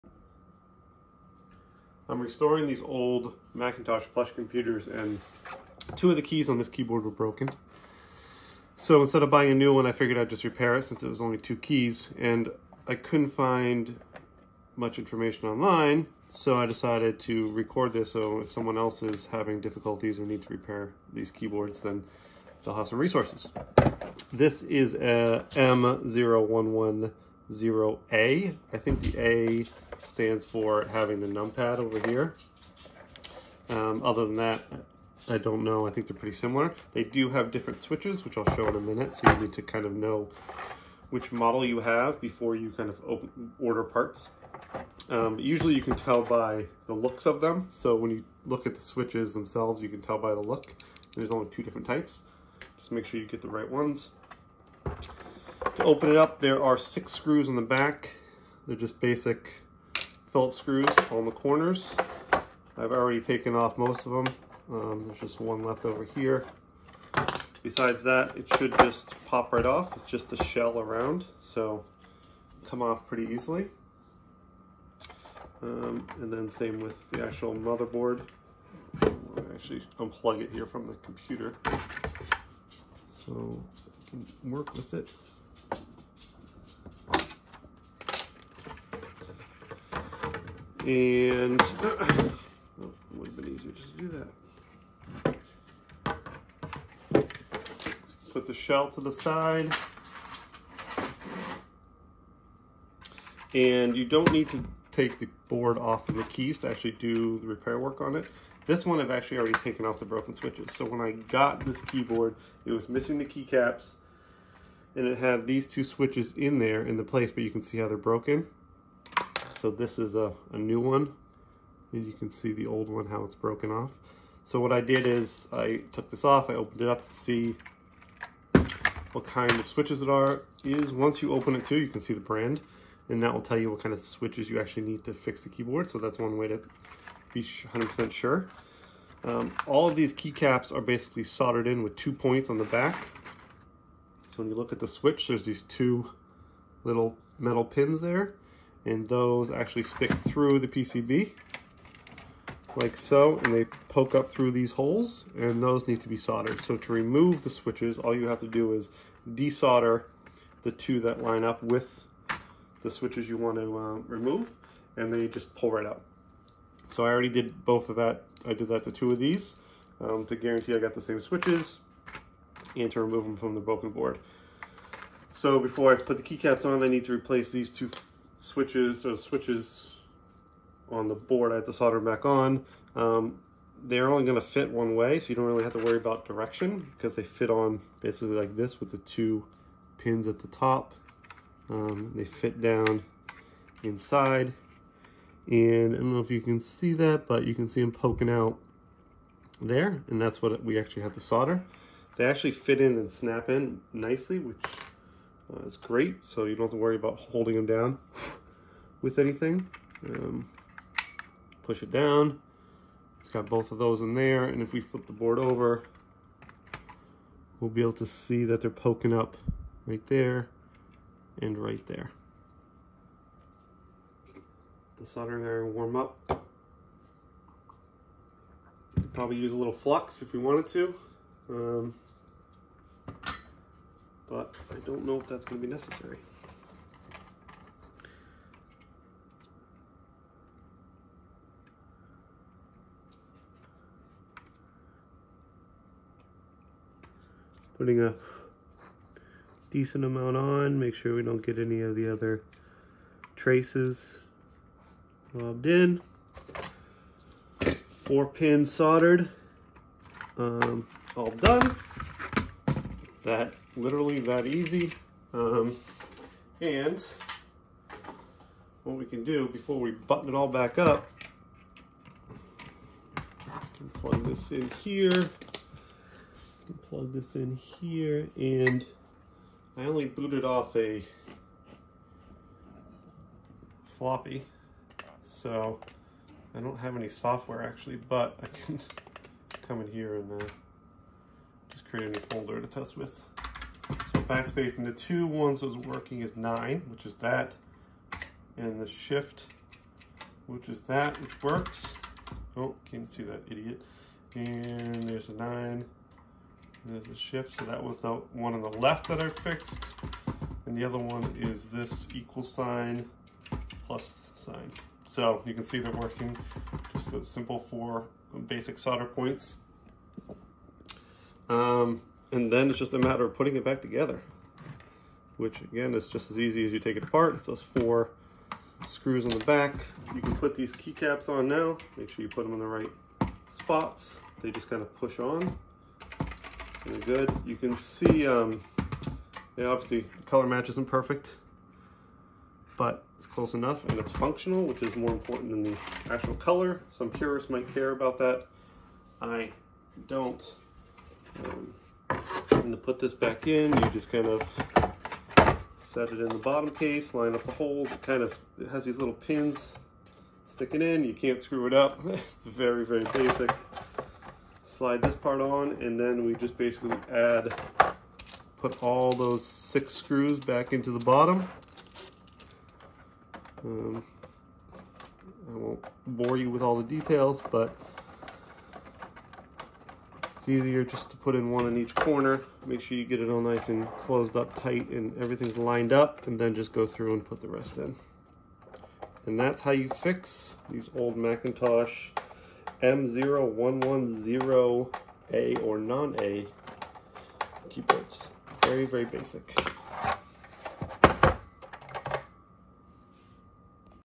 Macintosh Plus Keyboard Repair (M0110A)
How to replace keyboard switches on an old Macintosh Plus keyboard